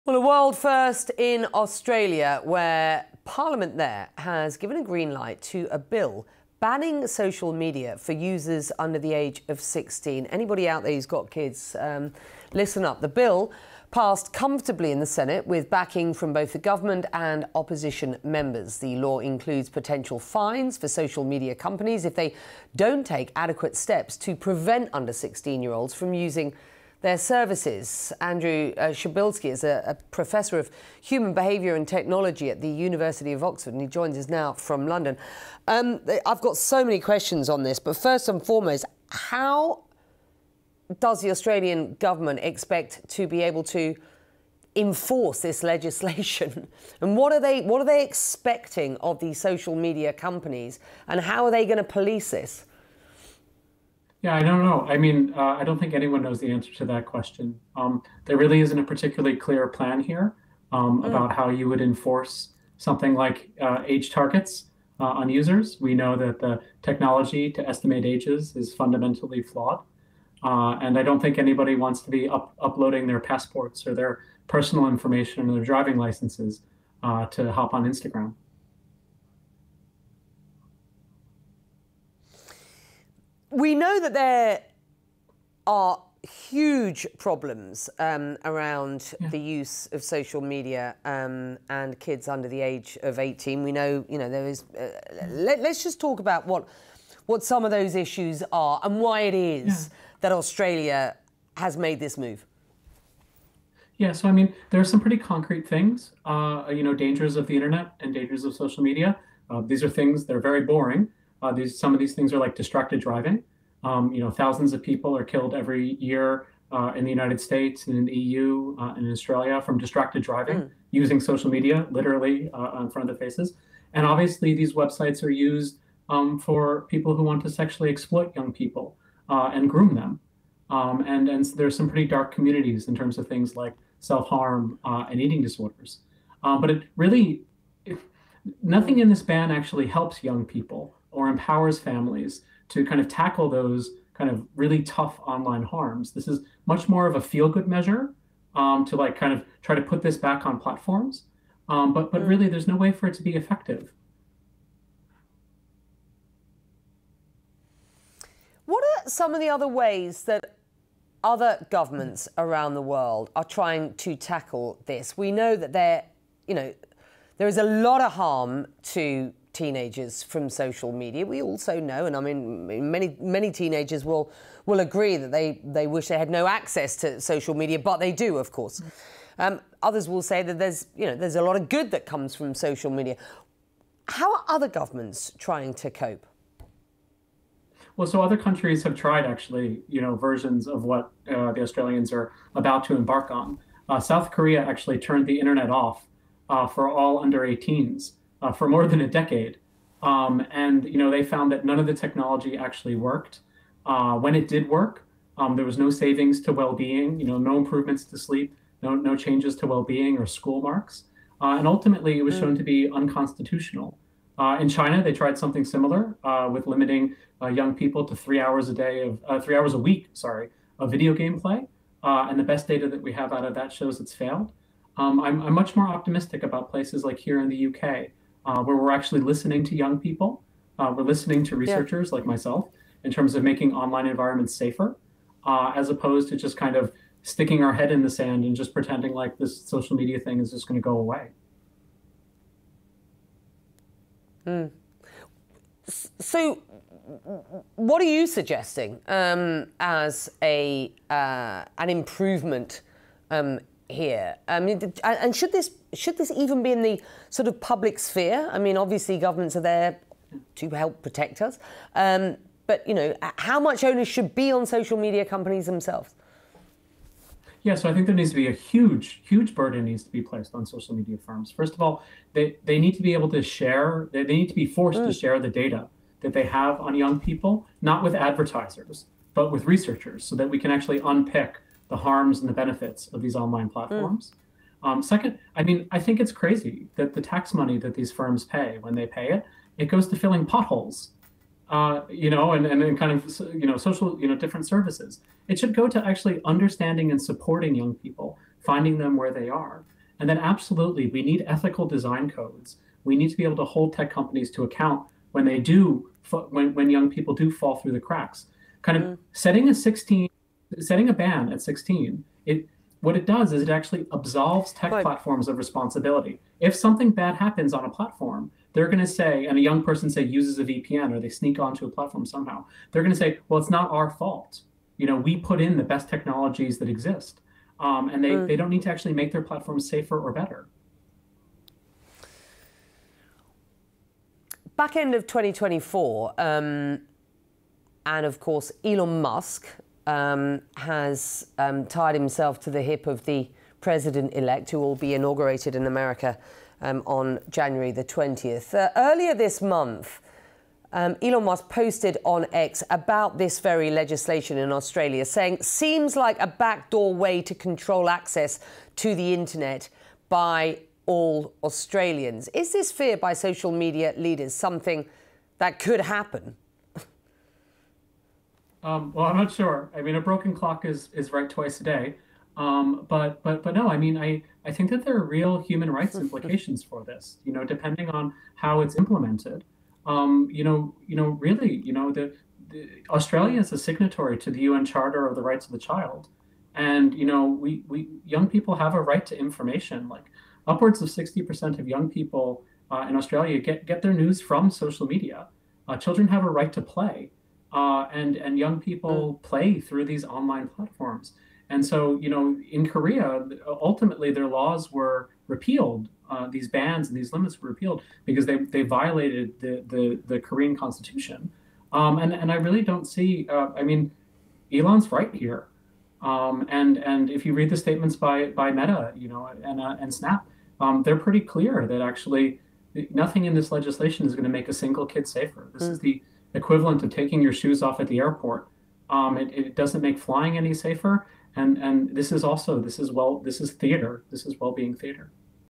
Expert explains why Australia's teen social media ban could do more harm than good
Australia’s parliament has passed a world-first law banning social media for children under 16. Andrew Przybylski, Professor of Human Behaviour and Technology at the University of Oxford joins CNN's Becky Anderson to discuss.